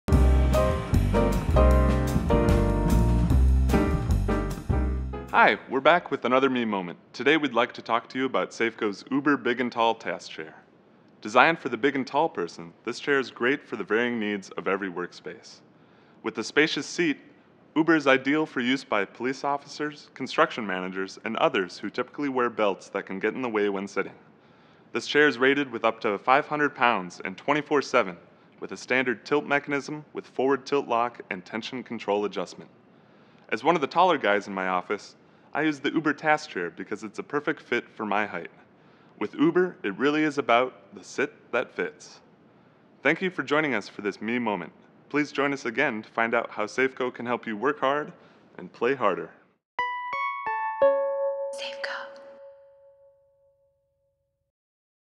Safco Uber Big and Tall Task Chair
When it comes to people, sizes vary. Variations are what makes us unique. Select the chair designed for all of life's varying shapes, sizes and working environments. Designed especially for the big and tall person, this chair offers a 500 lb. capacity. The Uber™ Big and Tall chair features a swivel seat with pneumatic height adjustment, tilt lock and tilt tension on a 5-star, over-sized base with dual hooded casters for added stability. Get the sit that fits!